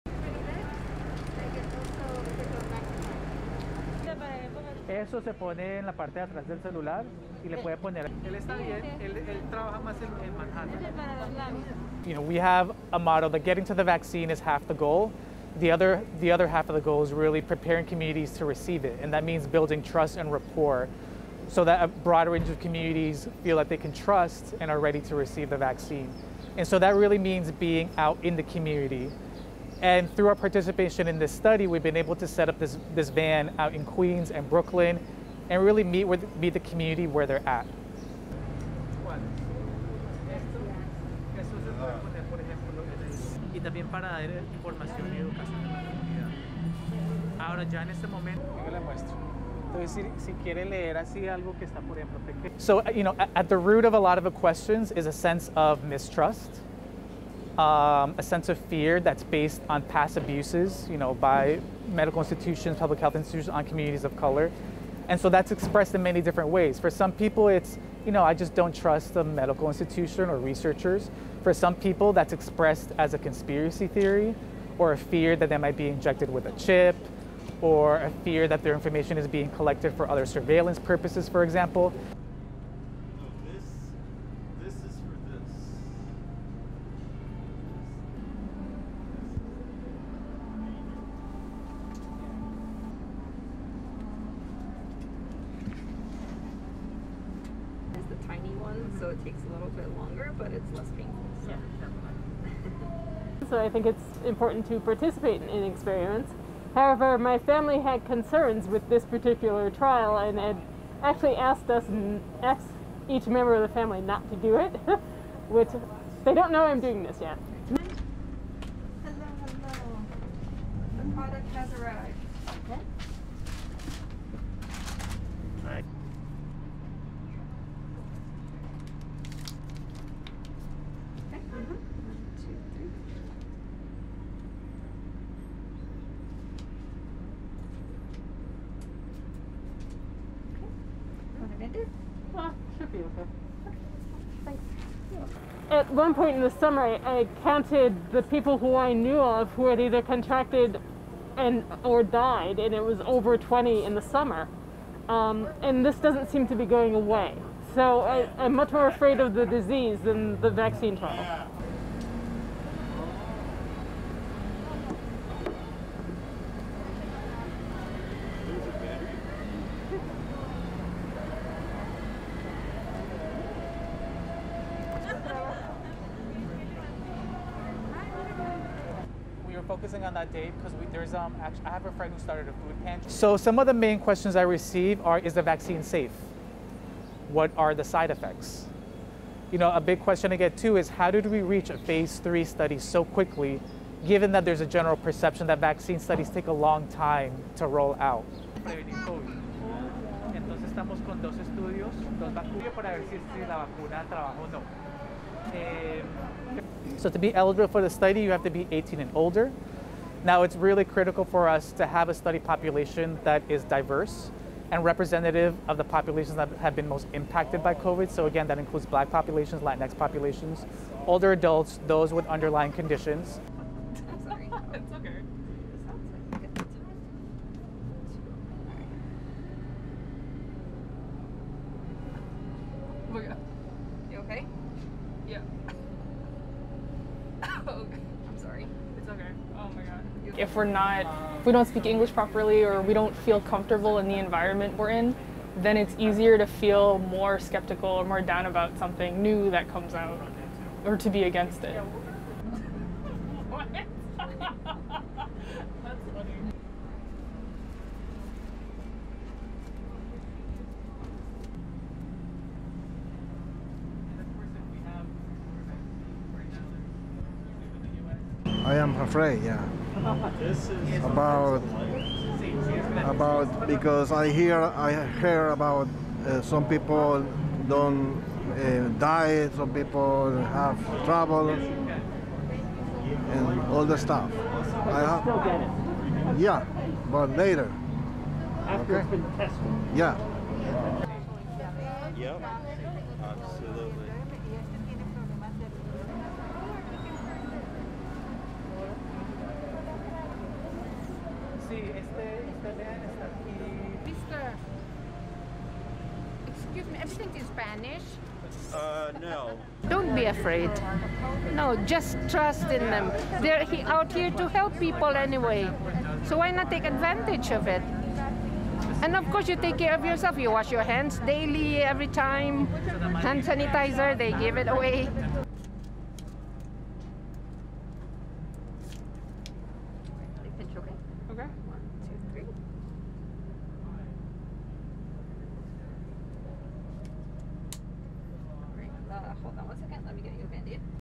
Van tours streets recruiting minorities for vaccine trials
(21 Jan 2021) US VACCINE DIVERSITY

RESTRICTION SUMMARY:
LENGTH: 5:41
ASSOCIATED PRESS
New York, US - 5 January 2021
1. Wide of Project Achieve van and display table, set up in Queens district to promote vaccine trials to minority communities
2. Close of man at display table
3. Mid of Project Achieve display
4. SOUNDBITE (English) Dr. Jorge Soler, Project Achieve co-investigator:
"You know, we have a motto that 'Getting to the vaccine is half the goal,' the other half of the goal is really preparing communities to receive it. And that means building trust and rapport so that a broad range of communities feel like they can trust and are ready to receive the vaccine. And so that really means being out in the community. And through our participation in this study, we've been able to set up this van out in Queens and Brooklyn and really meet the community where they're at."
5. Various of team speaking to people
6. COVID-19 educational material
7. SOUNDBITE (English) Dr. Jorge Soler, Project Achieve co-investigator:
"So, you know, at the root of a lot of questions is a sense of mistrust, a sense of fear that's based on past abuses by medical institutions, public health institutions on communities of color. And so that's expressed in many different ways. For some people it's, you know, I just don't trust the medical institution or researchers. For some people that's expressed as a conspiracy theory or a fear that might be injected with a chip or a fear that their information is being collected for other surveillance purposes, for example."
8. Various of Lani Muller having blood drawn as part of the AstraZeneca Phase 3 COVID-19 vaccine trial
9. SOUNDBITE (English) Lani Muller, vaccine trial participant:
"So I think it's important to participate in experiments. However, my family had concerns with this particular trial and actually asked us, each member of the family, not to do it, which they don't know I'm doing this yet."
10. Various of nurse giving Muller a dose of the trial vaccine
11. SOUNDBITE (English) Lani Muller, vaccine trial participant:
"At one point in the summer, I counted the people who I knew of who had either contracted and/or died, there was over 20 in the summer and this doesn't seem to be going away. So I'm much more afraid of the disease than the vaccine trial."
12. Muller walking out of the van, stopping at the outreach table
13. Person at the table
14. Postcard announcing vaccine trial
15. SOUNDBITE (English) Dr. Jorge Soler, Project Achieve co-investigator:
"So some of the main questions I receive are, is the vaccine safe? What are the side effects? You know, a big question I get too is 'How did we reach a Phase three study so quickly, given that there's a general perception that vaccine studies take a long time to roll out?'"
16. Dr. Soler talking to a man at the outreach table
17. Women asking a question
18. SOUNDBITE (English) Dr. Jorge Soler, Project Achieve co-investigator:
"So to be eligible for the study, you have to be 18 and older. Now, it's really critical for us to have a study population that is diverse and representative of the populations that have been most impacted by COVID. So, again that includes Black populations, Latinx populations, older adults, those with underlying conditions."
19. Various of Victorya Kaon being test for COVID-19 as part of the vaccine trial
20. SOUNDBITE (English) Victorya Kaon, vaccine trial participant:
21. Various of Kaon having blood drawn as part of the AstraZeneca Phase 3 COVID-19 vaccine trial
22. SOUNDBITE (English) Jamie Herrera, Manhattan resident:
REPORTER: "About what?"